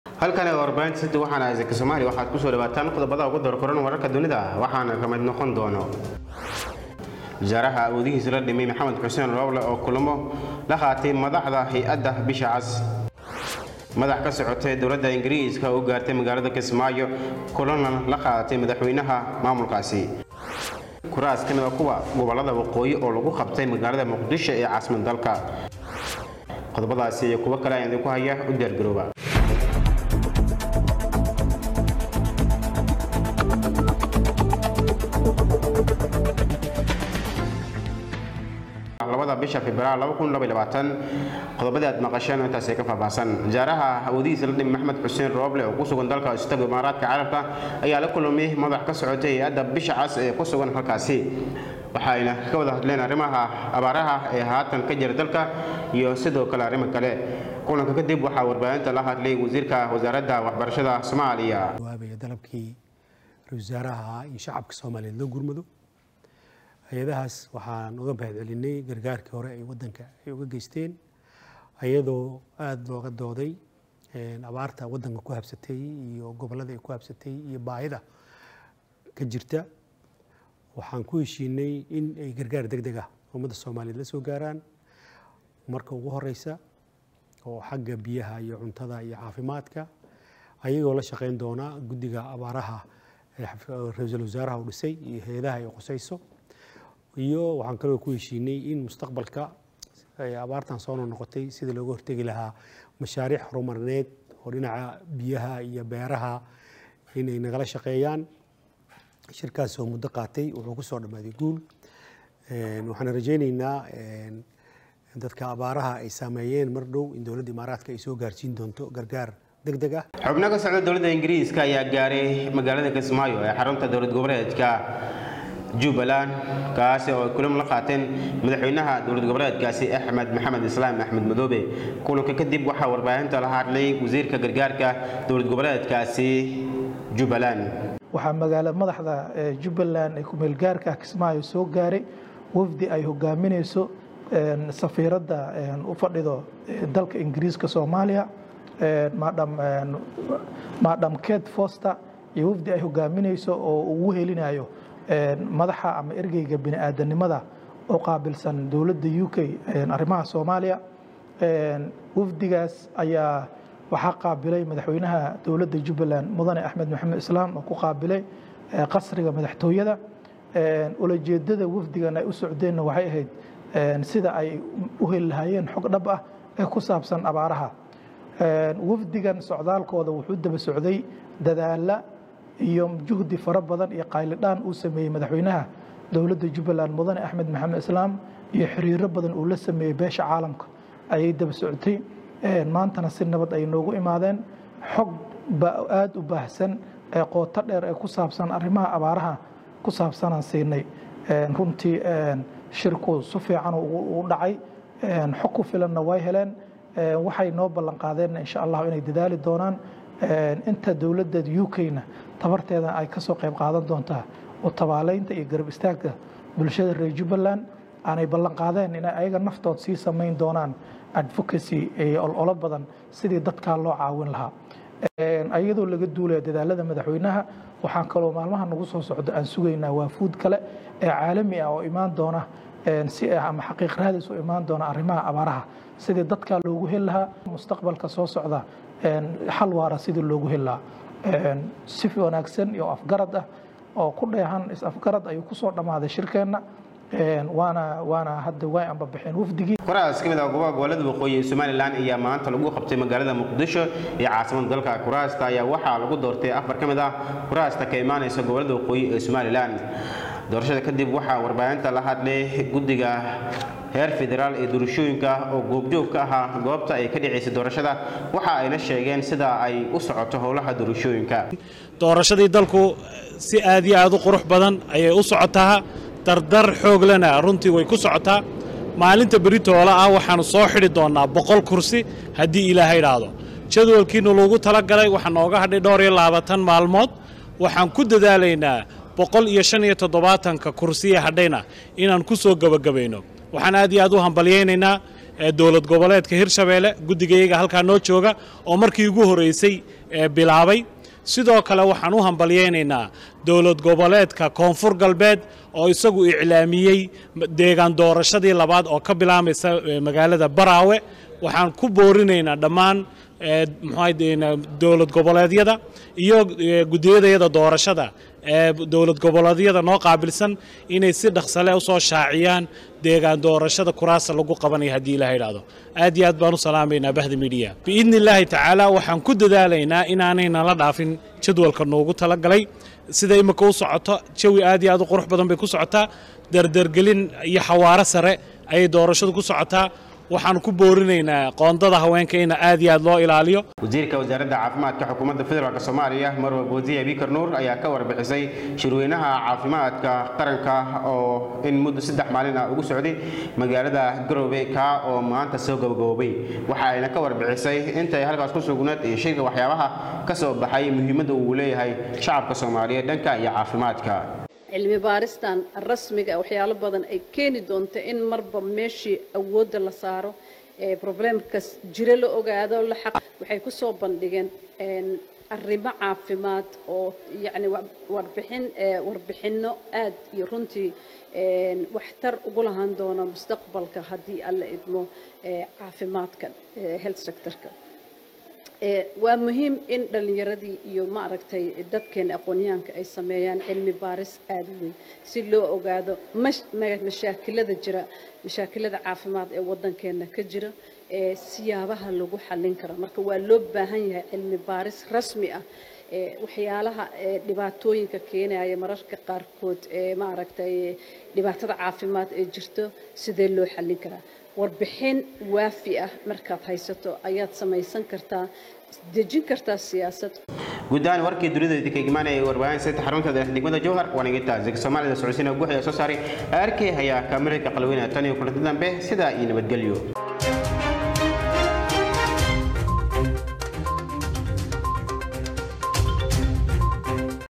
Wararka dunida iyo Caawa Qodobada ugu waaweyn Waxaa kamid ah, Xalaada Abaareed oo ka taagan
Wararka dunida iyo Caawa Qodobada ugu waaweyn Waxaa kamid ah, Xalaada Abaareed oo ka taagan Gobolada Dalka, Kullamada Rooble ee Wadanka Imaaraadka iyo Waxyaabihii kusoo kordhay Dagaalo ka dhacay, Gobolka Galgaduud.

Sakariye  ayaa Soo Jeedinaya